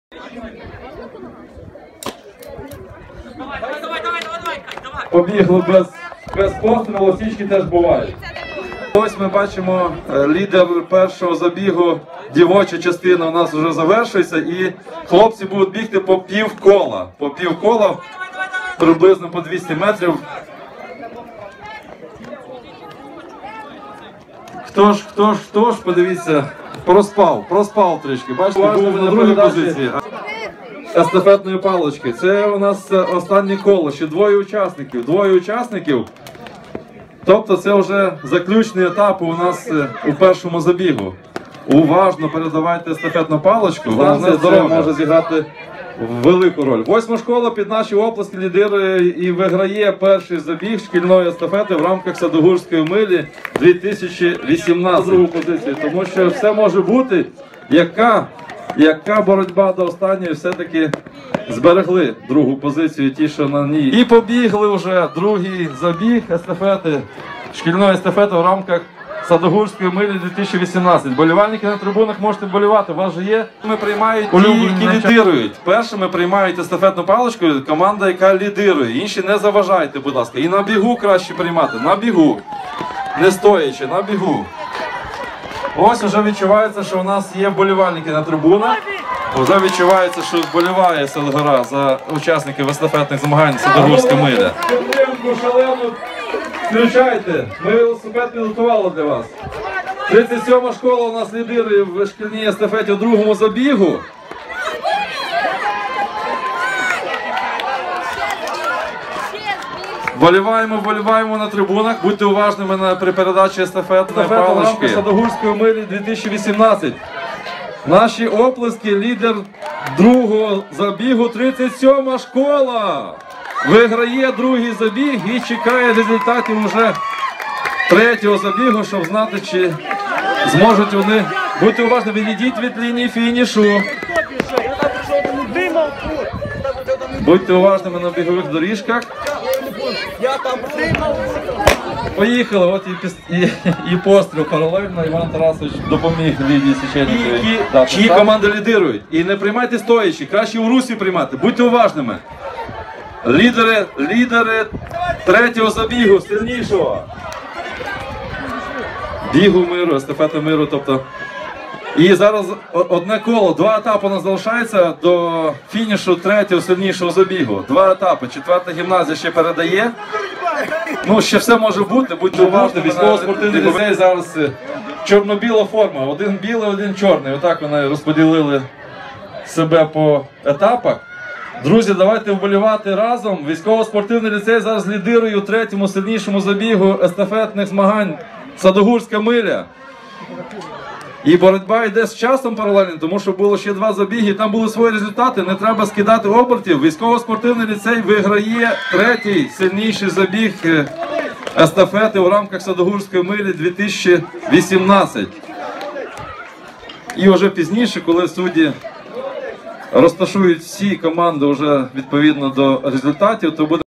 Садогурська миля 2018 (Шкільна естафета)
Чернівці, стадіон "Садгора", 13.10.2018 р.
ІІІ "Садогурська миля"
Шкільна естафета (3 забіги)
Відео: fakty.cv.ua (Георгій Мазурашу)
Партнер - СК "Гешко"